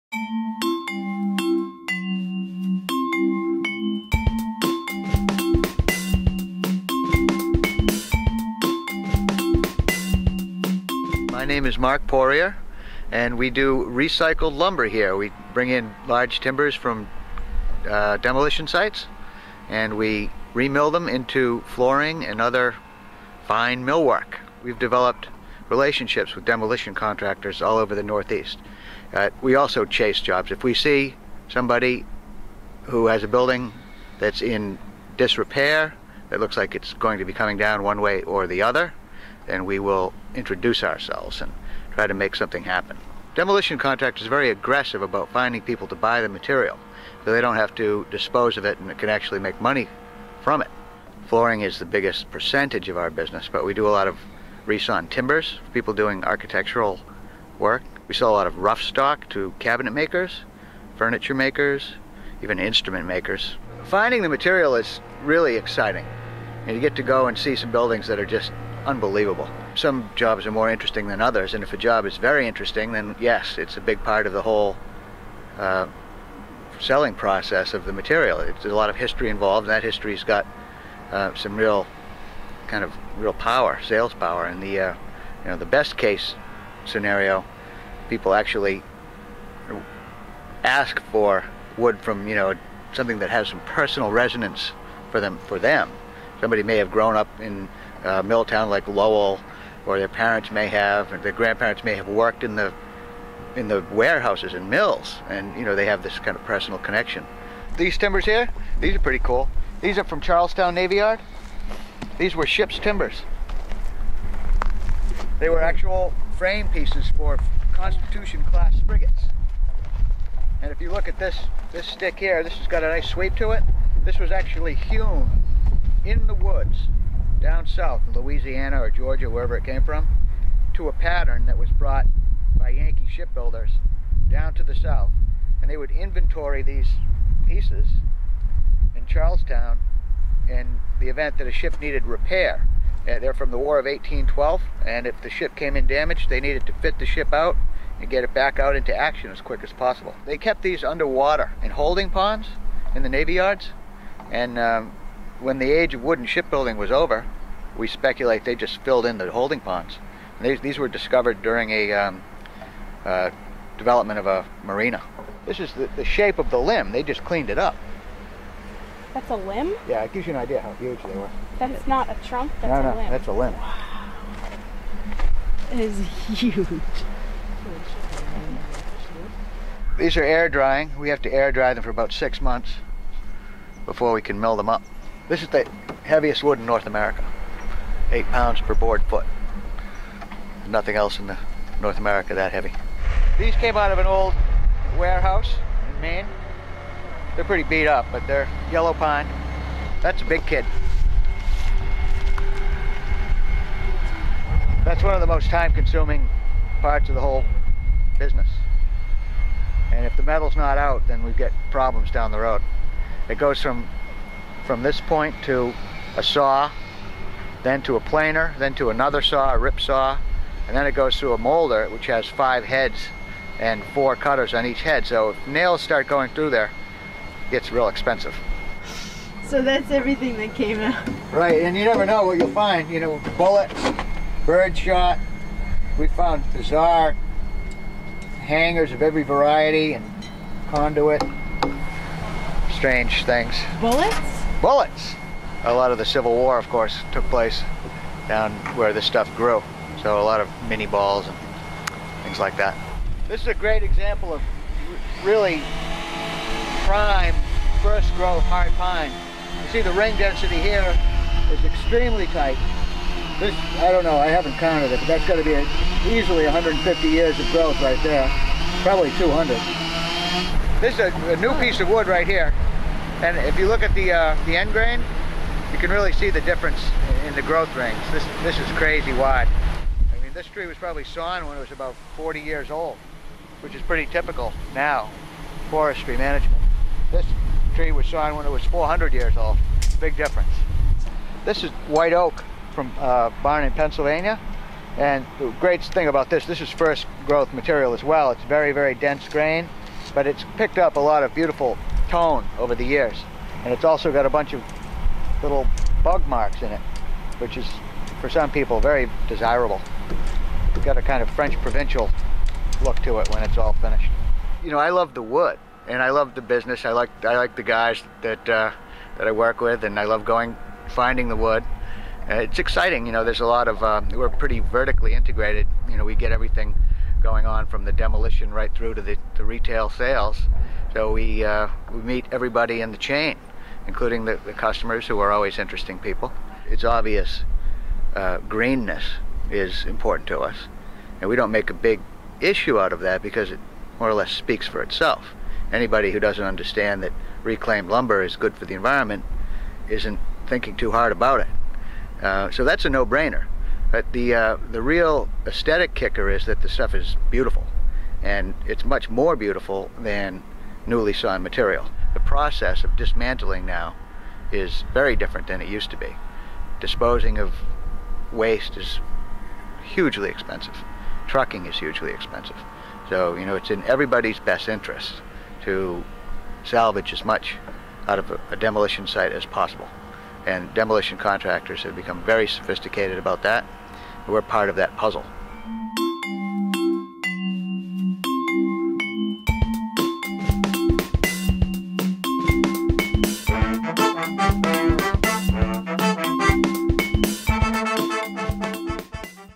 Longleaf Lumber- Reclaims More Than Just Wood
The ideas and business model behind Longleaf Lumber tickle our fancy in more ways than one. By reclaiming antique wood from demolition sites, Marc Poirier is the ultimate recycler. Like an archeologist, Marc delivers the history and stories behind almost every timber in his Berwick, Maine stock. Look closely and you'll easily see the difference between a 400 year old limb and a newly sawn board from a warehouse store. Special thanks to Ashley and Kyra from SustainableRoute.com for production support. Check out LongleafLumber.com.